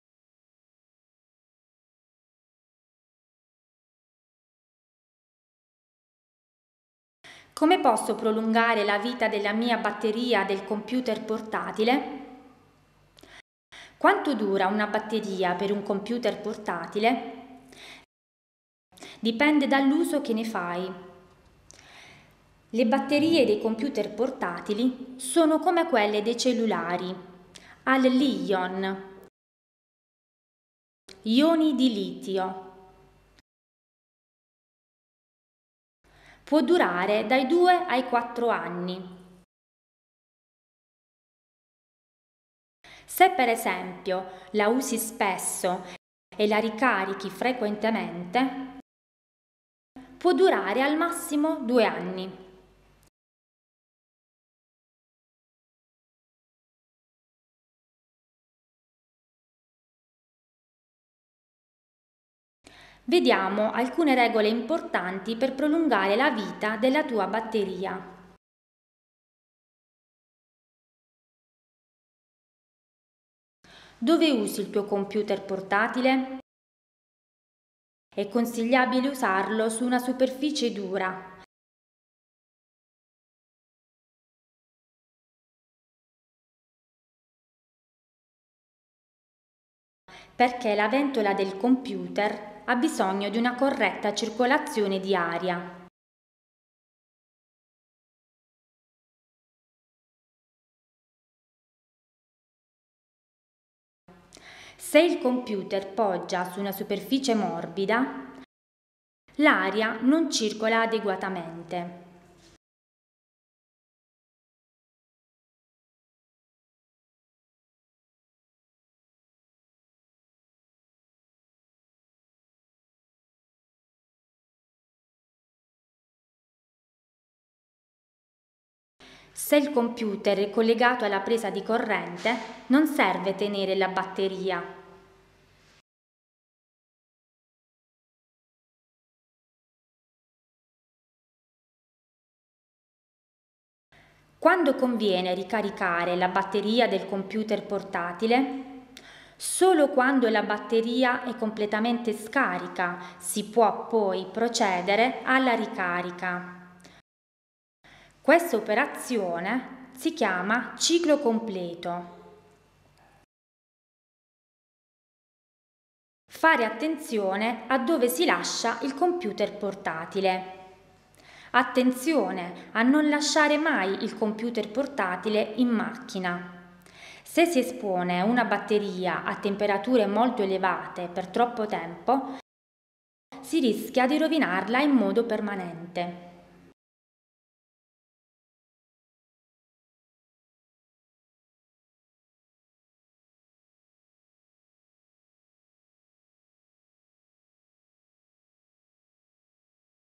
La batteria del computer
Mimma ci spiega in lingua dei segni italiana cos'è e come è fatta la batteria di un computer portatile come fare per farla durare il più possibile.